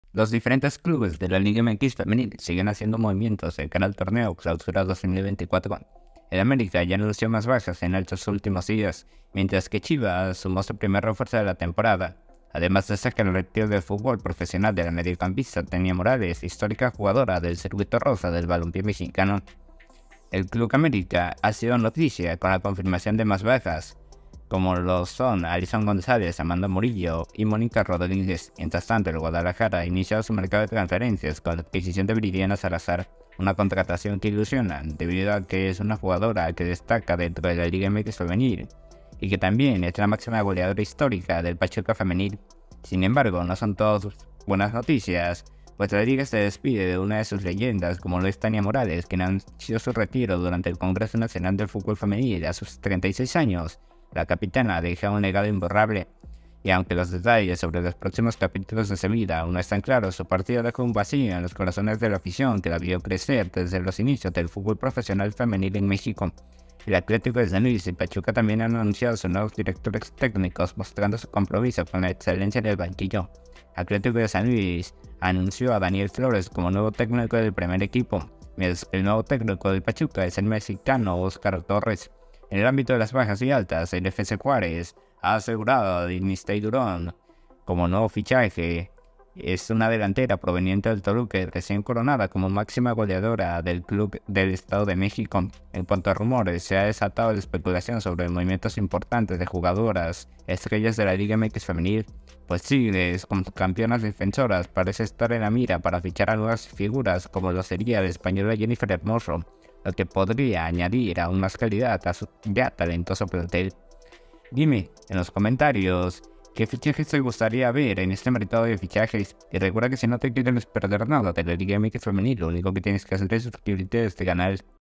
BAJAS Y ALTAS CONFIRMADAS | LIGA MX FEMENIL
Los diferentes clubes de la Liga MX Femenil siguen haciendo movimientos de cara al torneo Clausura 2024. El América ya anunció más bajas en estos últimos días, mientras que Chivas sumó su primer refuerzo de la temporada. Además, destaca el retiro del futbol profesional de la mediocampista Tania Morales, histórica jugadora del circuito rosa del balompié mexicano quien puso fin a su carrera esta temporada.

El Club América ha sido noticia con la confirmación de más bajas, incluyendo figuras como Alison González, Amanda Murillo y Mónica Rodríguez.
Mientras tanto, el Guadalajara ha iniciado su mercado de transferencias con la adquisición de Viridiana Salazar,  una contratación que ilusiona al equipo debido a que es una jugadora destacada en la liga MX femenil y que tambien es la maxima goleadora del Pachuca Femenil.

Sin embargo, no todo son altas y buenas noticias. La Liga se despide de una de sus leyendas, Tania Morales, quien anunció su retiro durante el Congreso Nacional de Fútbol Femenil. A sus 36 años, la 'Capitana' deja un legado imborrable y, aunque los detalles sobre los próximos capítulos de su vida  aún no están claros, su partida deja un vacío en los corazones de la afición que la vio crecer desde los inicios del fútbol profesional femenil en México.

El Atlético San Luis y Pachuca también han anunciado a sus nuevos directores técnicos, mostrando su compromiso con la excelencia en el banquillo.
Atletico de San Luis anuncia a Daniel Flores como nuevo tecnico del primer equipo
Mientras que el nuevo tecnivo del Pachuca es el mexicano  Óscar Torres

En el ámbito de las bajas y altas, el FC Juárez ha asegurado a Destinney Durón, una delantera proveniente del Toluca y recién coronada como maxima goleadora del Club del Estado de Mexico

En cuanto a rumores, se han desatado las especulación sobre movimientos importantes de jugadoras estrella de la Liga MX Femenil. Tigres, como campeonas defensoras, parece estar en la mira para fichar a nuevas figuras como la española Jennifer Hermoso, lo que podría añadir aún más calidad a su ya talentoso plantel.

Imagenes y videos mostrados, no son de nuesta autoria. Creditos totales para los dueños de los mismos.